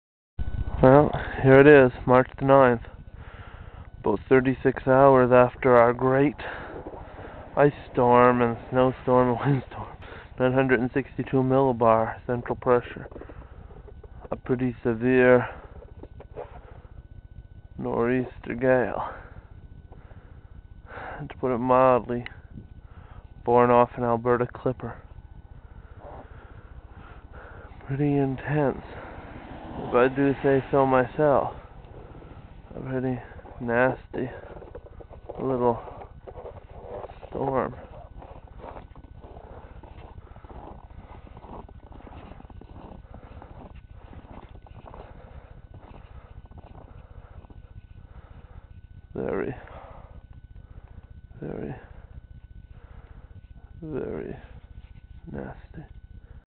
March 2005 Ice Storm in New Brunswick, Canada
Check out this intense ice accretion - up to 3 inches or 7.5 cm on some surfaces including trees.